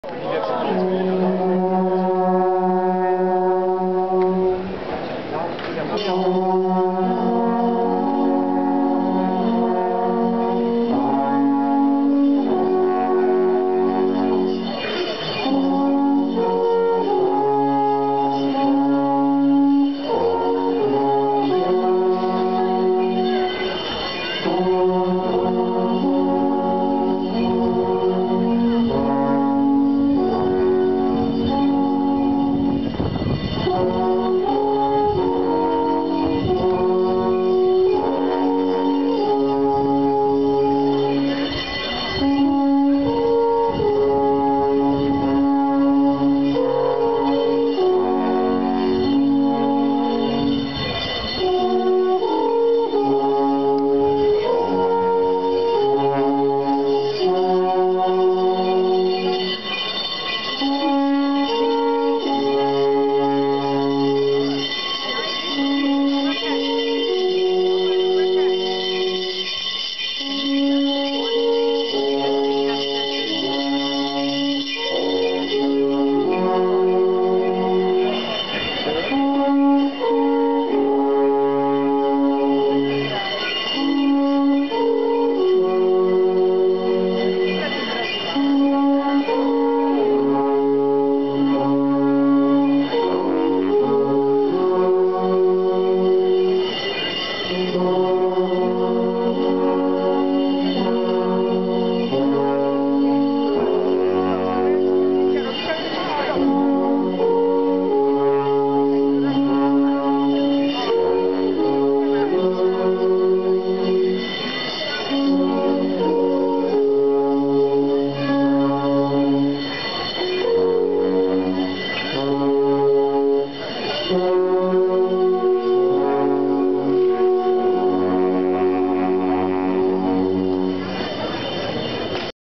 Schweizer Folklore Festival 5 Das Alphorn Sechstet
In Zermatt wird gefeiert und sich verkleidet!